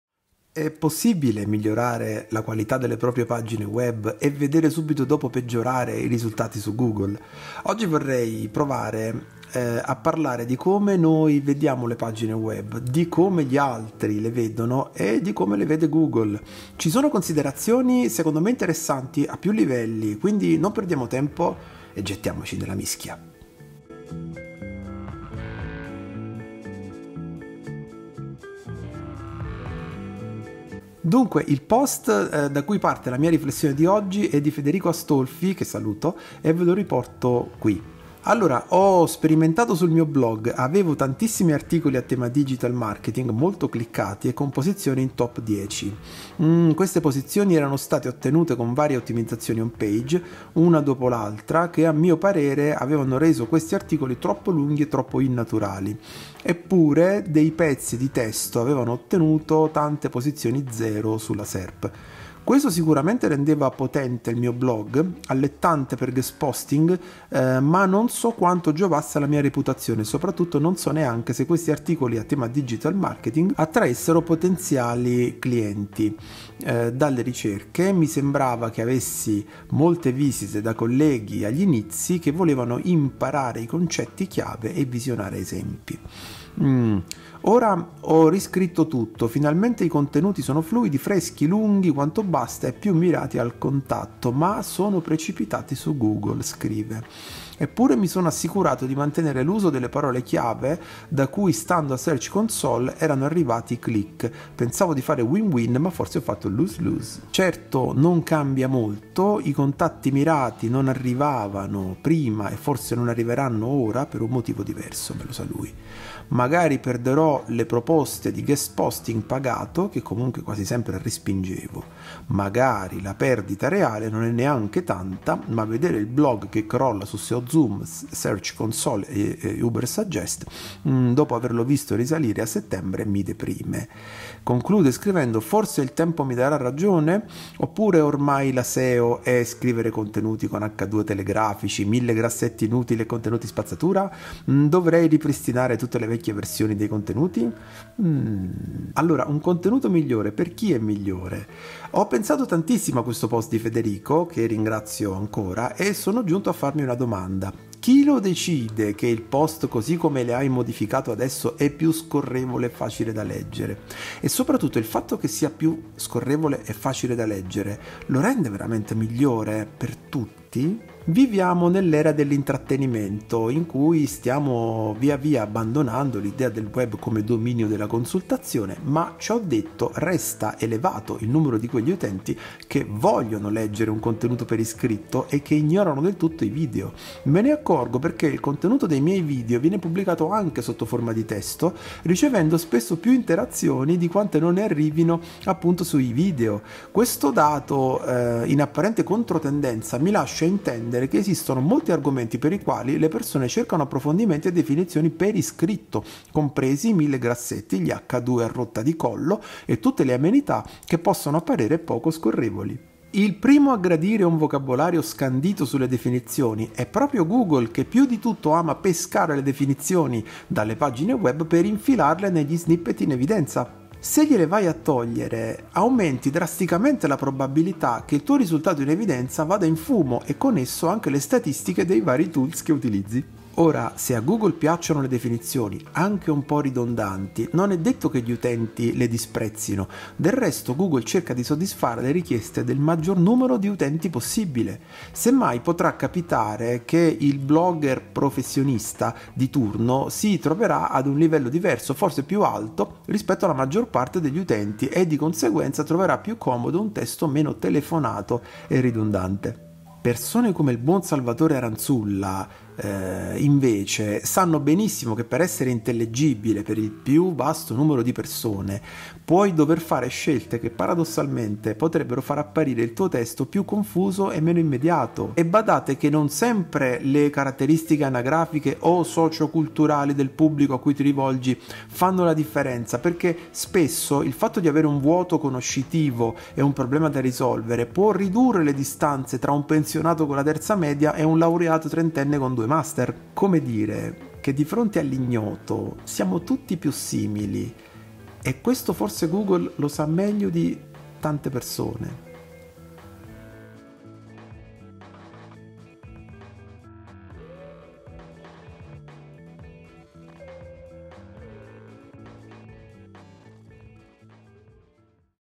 Perché i contenuti più scorrevoli si posizionano peggio su Google?
Ho migliorato la leggibilità dei contenuti, ma ho perso visibilità su Google
Mille grassetti e definizioni ridondanti, peggiorano davvero l'esperienza di lettura? E per chi? Non sarà che rendendo un testo più "scorrevole" e privo di tutte quelle definizioni che piacciono a Google, andiamo a peggiorare di fatto la qualità del contenuto per la maggior parte degli utenti?
Non sarà che "la maggior parte" degli utenti spesso reagisce diversamente al testo rispetto a chi lo produce?
E se Google sapesse più cose su di noi, di quante noi stessi crediamo di conoscerne?

00:00 🔍 Migliorare la qualità delle pagine web non garantisce un miglior posizionamento su Google; la percezione di "miglioramento" varia tra utenti e motori di ricerca.
00:54 ✍️ Riscrivere articoli per renderli più scorrevoli può far perdere posizioni su Google, anche mantenendo l'uso delle stesse parole chiave.
02:20 🤔 L'idea che contenuti più scorrevoli siano automaticamente migliori è discutibile; Google e gli utenti hanno esigenze diverse.
03:13 📜 Google apprezza definizioni e contenuti strutturati con H2 e grassetti, spesso utilizzandoli per snippet in evidenza.
04:20 🧩 Scrivere per un vasto pubblico può richiedere scelte che sembrano controintuitive, come semplificare o rendere ridondanti alcune informazioni.
04:49 🌍 Di fronte all'ignoto, le differenze tra utenti si riducono; Google potrebbe essere consapevole di questo e strutturare i suoi risultati di conseguenza.